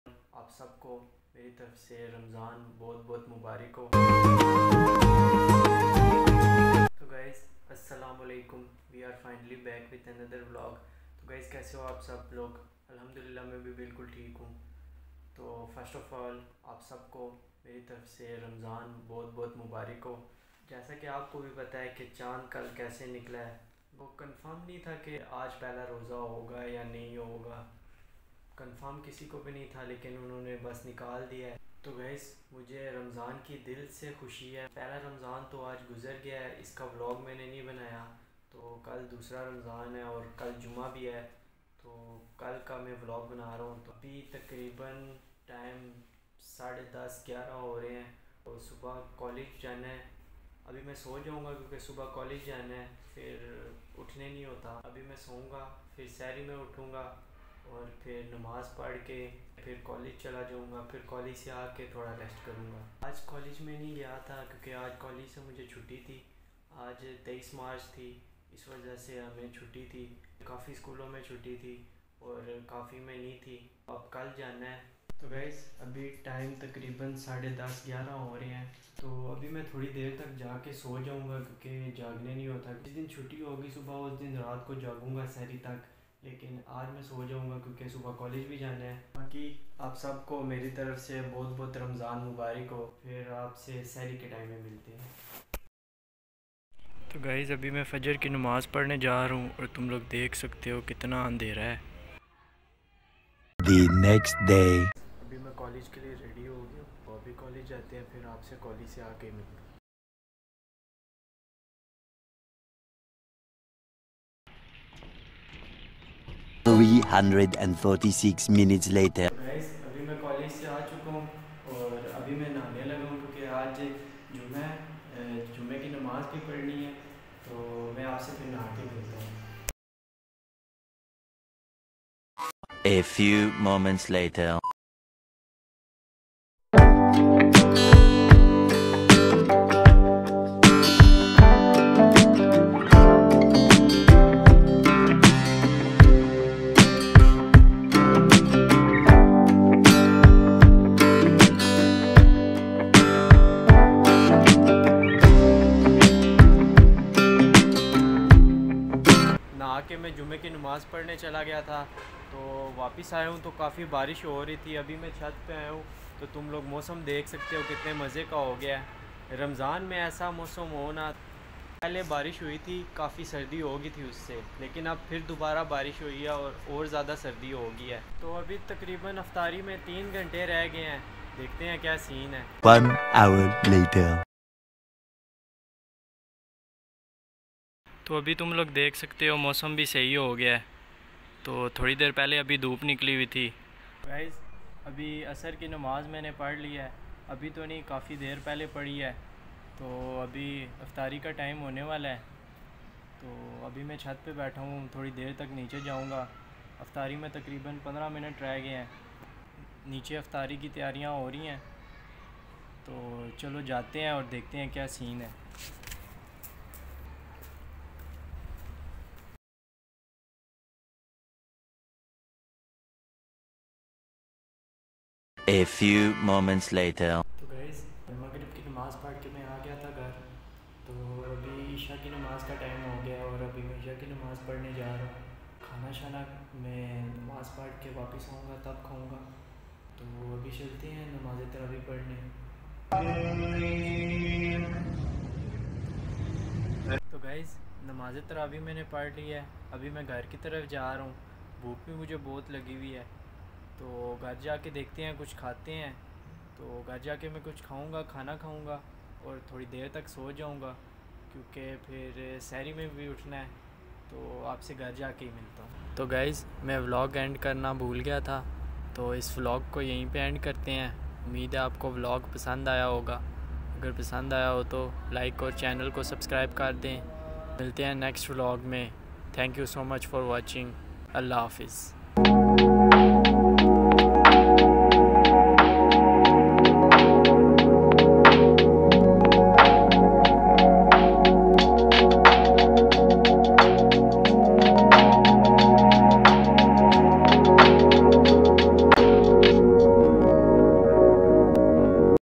Ramadan Mubarak 😍 | Day 02 | Ramadan 2023
Ramadan Mubarak 😍
Ramadan 2023
2nd Ramadan

•Sharing my Ramadan Routine with you Guy's